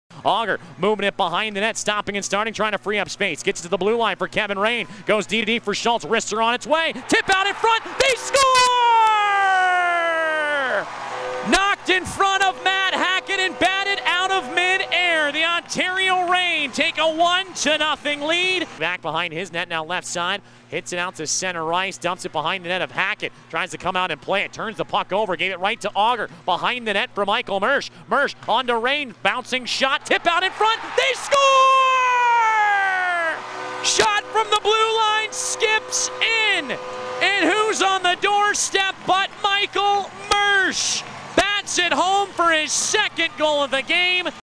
Michael Mersch scores twice in Reign playoff win - 05/05/16
Michael Mersch powered the Ontario Reign past the San Diego Gulls in game one of their second round Calder Cup playoff series.